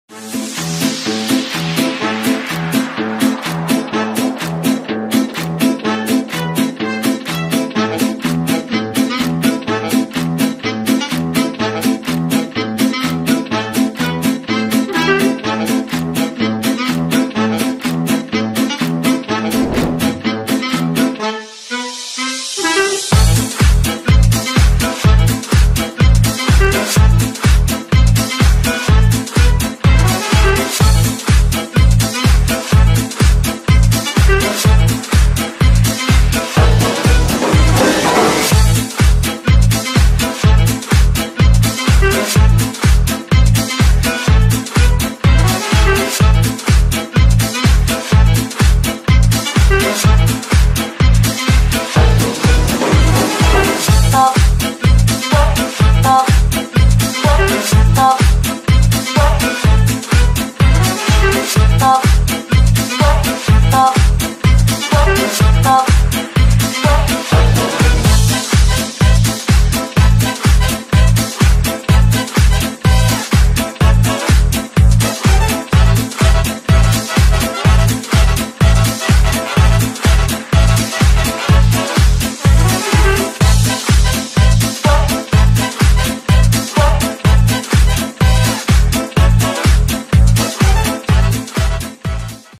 Beirut - Cả một bầu trời Trung Âu toạ lạc ngay tại trung tâm Sài Gòn | Địa điểm ăn uống
Beirut - Mediterranean Restaurant & Shisha Lounge
Địa chỉ: 21 Ngô Đức Kế, P. Bến Nghé, Q.1
Thời gian: 11:00 - 1:00